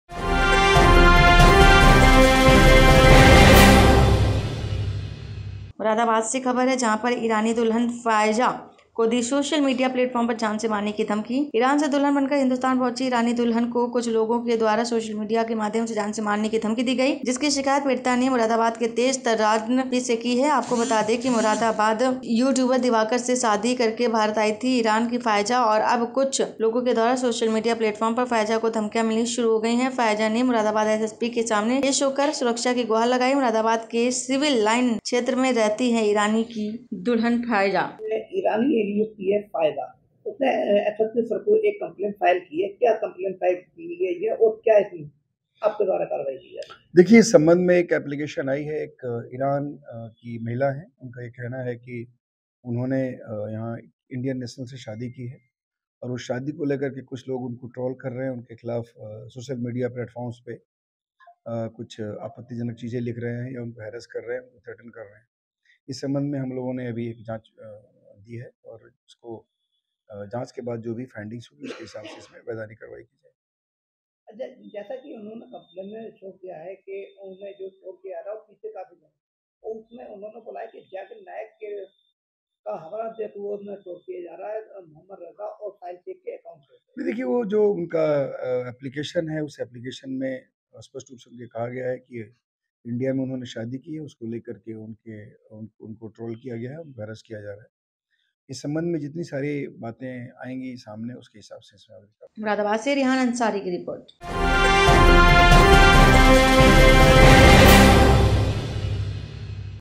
Moradabad : ईरानी दुल्हन फ़ायजा को दी सोशल मीडिया प्लेटफॉर्म पर धमकी। JANKANPUR24X7
Your Queries:-ईरान से दुल्हन बनकर हिंदुस्तान पहुंची ईरानी दुल्हन को कुछ लोगो के द्वारा सोशल मीडिया के माध्यम से जान से मारने की धमकी दी गई है जिसकी शिकायत पीड़िता ने मुरादाबाद के तेज़तर्रार एसएसपी से की है आपको बता दे कि मुरादाबाद के युट्यूबर दिवाकर से शादी करके भारत आई थी ईरान की फ़ायजा अब कुछ लोगो के द्वारा सोशल मीडिया प्लेटफॉर्म पर फ़ायजा को धमकियां मिलनी शुरू हो गई फ़ायजा ने मुरादाबाद एसएसपी के सामने पेश होकर सुरक्षा की गुहार लगाई मुरादाबाद के सिविल लाइन क्षेत्र मैं रहती है ईरानी दुल्हन फ़ायजा |